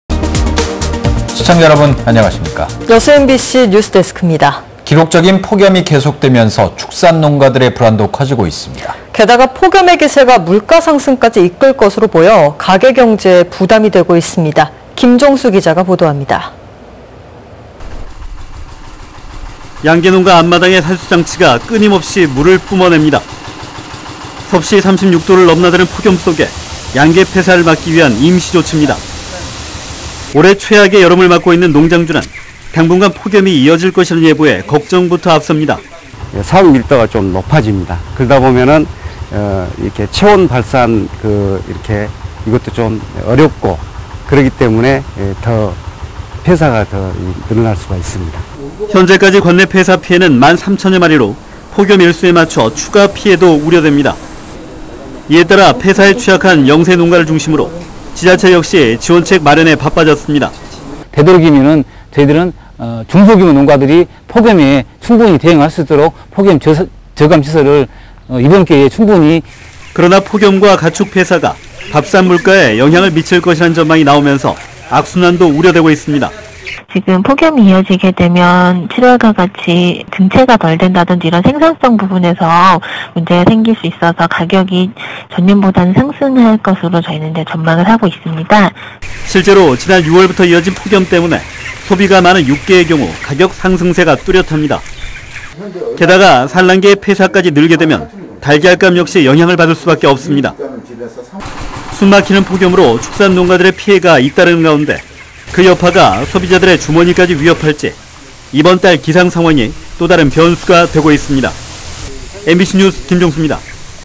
가축폐사.. 물가상승 우려 - R (180802목/뉴스데스크)
역대 최악의 폭염으로 축산농가들의 불안이 커지고 있습니다.
게다가 폭염의 기세가 물가상승까지 이끌 것으로 보여
가계경제에 부담이 되고 있습니다.

김종수 기자가 보도합니다.
◀ＶＣＲ▶
양계농가 앞마당의 살수장치가
끊임없이 물을 뿜어냅니다.

섭씨 36도를 넘나드는 폭염 속에
양계 폐사를 막기 위한 임시 조치입니다.

올해 최악의 여름을 맞고 있는 농장주는
당분간 폭염이 이어질 것이란 예보에
걱정부터 앞섭니다.
◀ＩＮＴ▶
주동하(순천시 별량면) :
(성장하면서) 사육밀도가 높아집니다. 그렇다 보면 체온발산이 어렵고 그렇기 때문에 폐사가 더 늘어날 수가 있습니다.

현재까지 관내 폐사피해는 만 3천여 마리로
폭염일수에 맞춰 추가피해도 우려됩니다.

이에 따라 폐사에 취약한 영세농가를 중심으로
지자체 역시 지원책 마련에 바빠졌습니다.
◀ＩＮＴ▶
김찬호 팀장(순천시 축산팀) :
되도록이면 저희들은 중소규모 농가들이 폭염에 충분히 대응할 수 있도록 폭염저감시설을 이번 기회에 충분히 (지원하겠습니다.)

그러나 폭염과 가축폐사가
밥상물가에 영향을 미칠 것이란 전망이
나오면서 악순환도 우려되고 있습니다.
◀ＳＹＮ▶
심민희 연구원(한국농촌경제연구원) :
지금 폭염이 이어지게 되면 7월과 같이 증체가 덜 된다든지 생산성 부분에서 문제가 생길 수 있어서 가격이 전년보다 상승할 것으로 전망하고 있습니다.

실제로 지난 6월부터 이어진 폭염 때문에
소비가 많은 육계의 경우 가격 상승세가
뚜렷합니다.

게다가 산란계의 폐사까지 늘게 되면
달걀값 역시 영향을 받을 수밖에 없습니다.

숨막히는 폭염으로
축산농가들의 피해가 잇따르는 가운데
그 여파가 소비자들의 주머니까지 위협할지
이번 달 기상상황이
또 다른 변수가 되고 있습니다.

MBC뉴스 김종수입니다.
◀ＥＮＤ▶